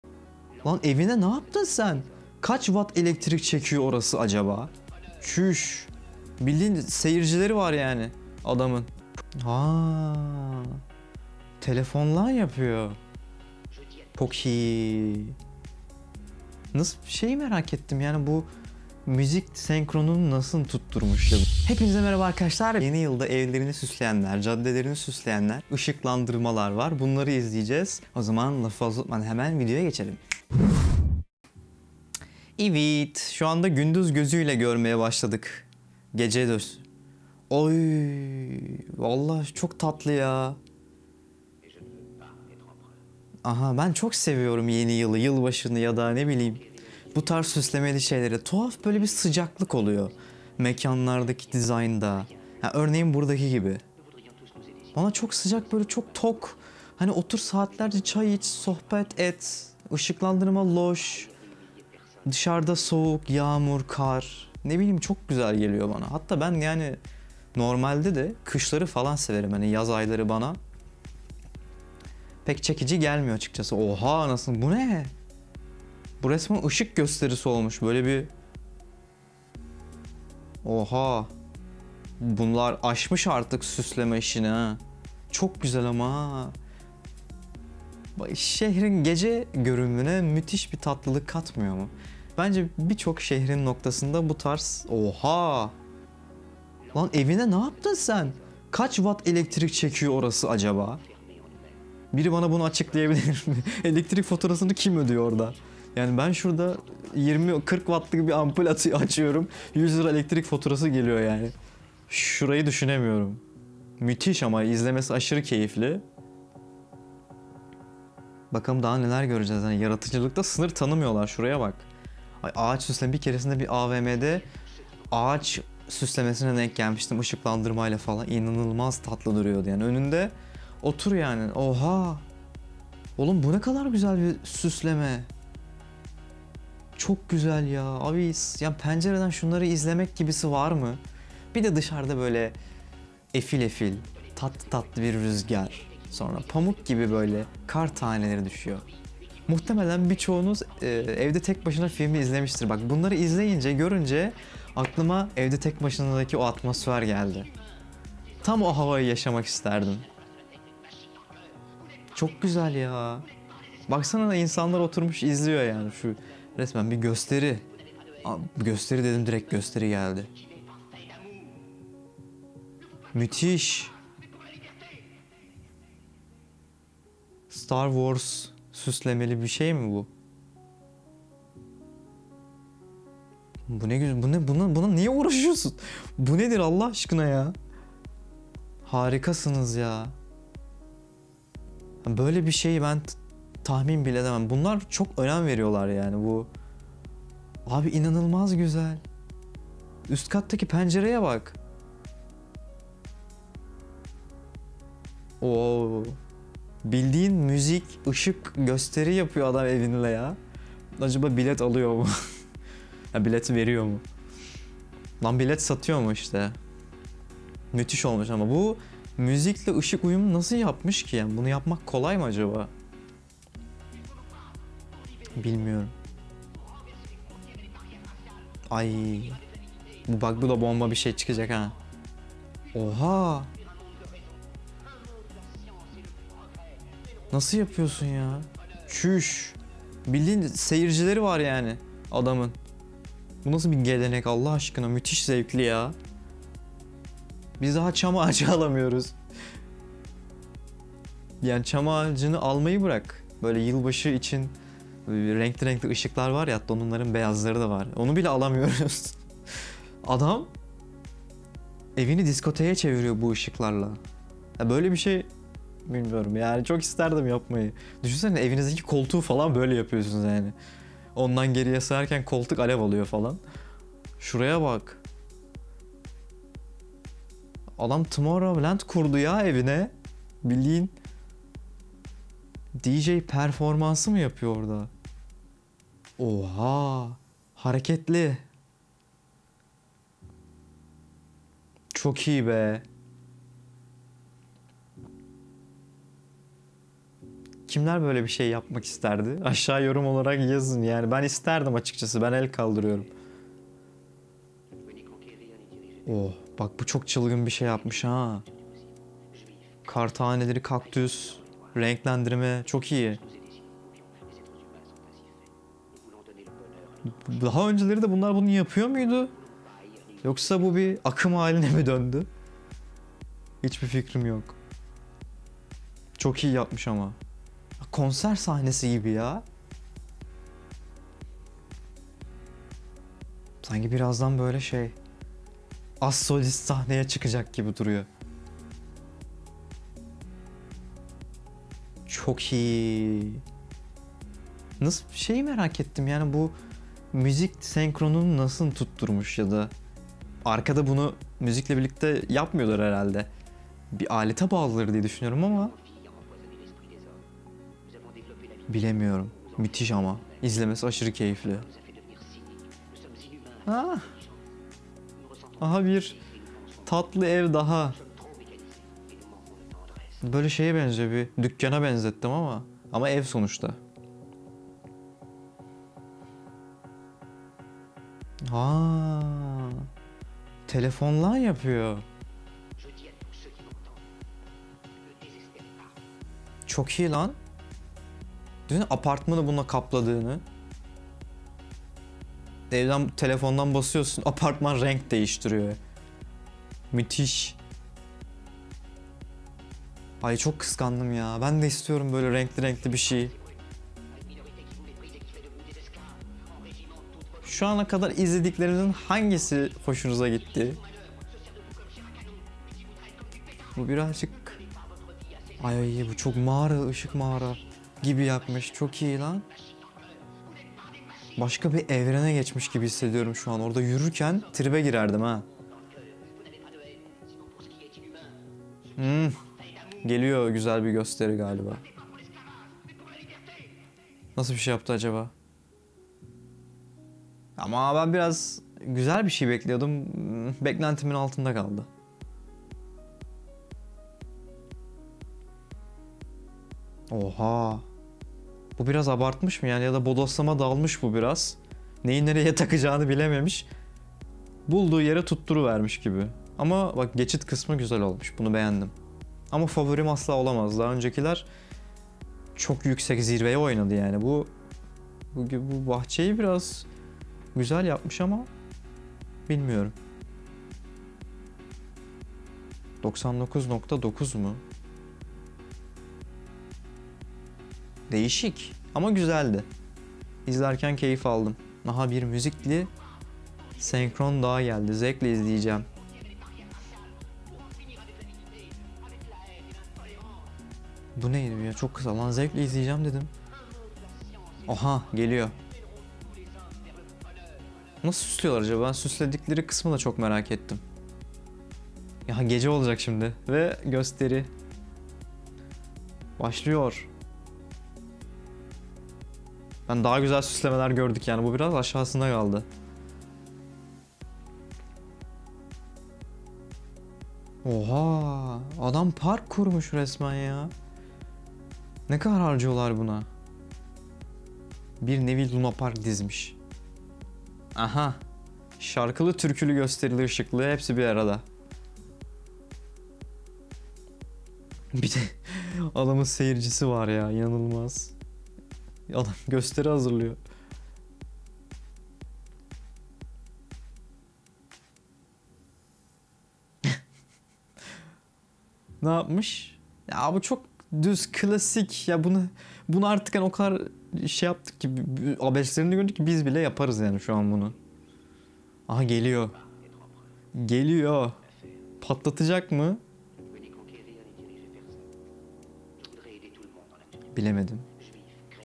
Yılbaşı Süslemeleri Yapan İnsanların Işıklı Evleri
Yeni yıl yada yılbaşı geldi çattı 2021 senesine girmek için hazırlanan insanların evlerini nasıl süslediklerini izliyoruz. Işıklarla evlerini süsleyen insanların videolarını izliyoruz.

Beni Sosyal Medyadan Takip Et!
Benim adım Serdar, sanırım bu işi yapmayı çok seviyorum.